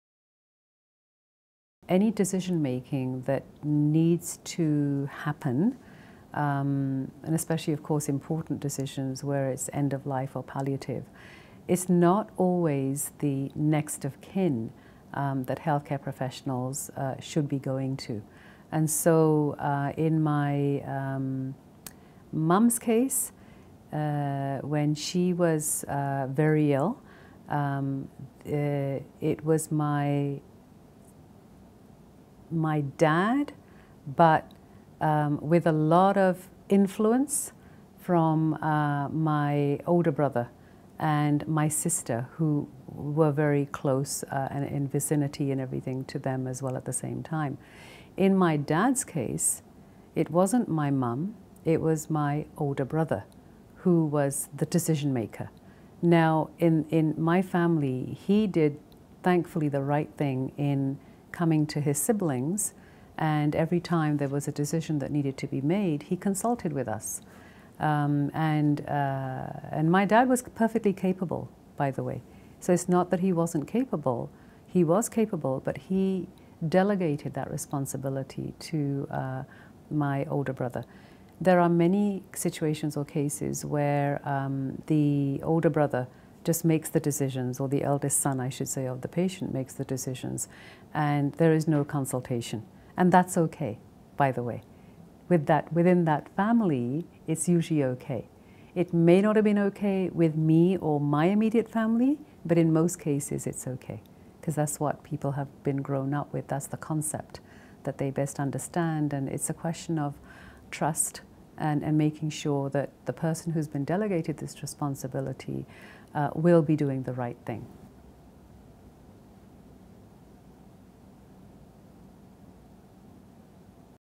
Gurwinder: Who is the decision maker?
In this video by Canadian Virtual Hospice, Gurwinder speaks about how persons in the Indian culture communicate when there are healthcare decisions to be made.